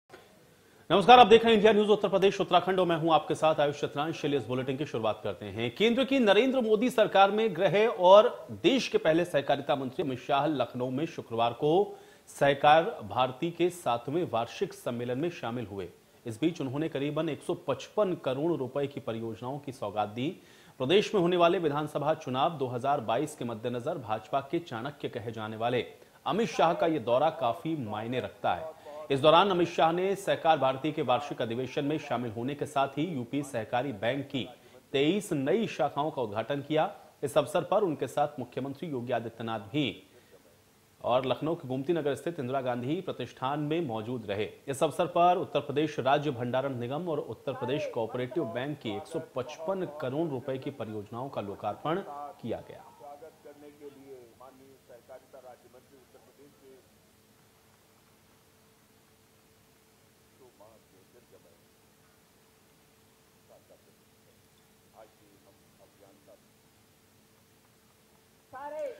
केंद्रीय गृहमंत्री अमित शाह ने योगी सरकार को कहा- किसानों का भला करने वाली सरकार
केंद्रीय गृहमंत्री अमित शाह ने योगी सरकार को कहा- किसानों का भला करने वाली सरकार | India News UPUK